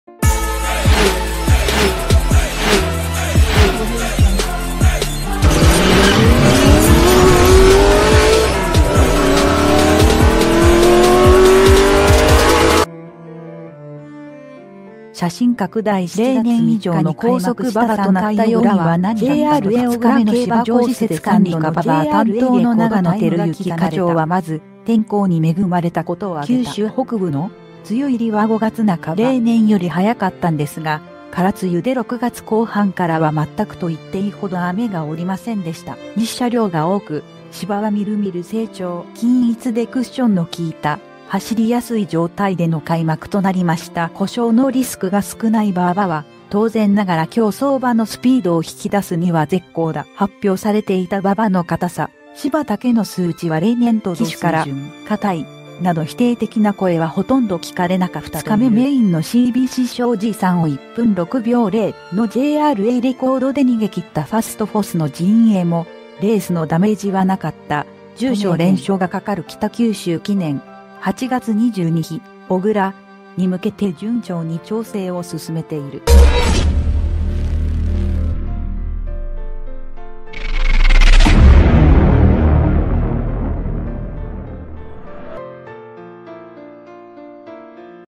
ＪＲＡレコードが連発した７月の小倉 「走りやすい」条件がそろった芝は例年以上に高速化|Top News
ＪＲＡレコードが連発した７月の小倉 「走りやすい」条件がそろった芝は例年以上に高速化|Top News 写真拡大 ７月３日に開幕した３回小倉は、初日と２日目の芝コースで３度のＪＲＡレコードタイムが刻まれた。例年以上の高速馬場となった要因は何だったのか。 ＪＲＡ小倉競馬場施設管理課・馬場担当の長野照幸課長はまず、天候に恵まれたことを挙げた。「（九州北部の）梅雨入りは５月半ば。例年より早かっ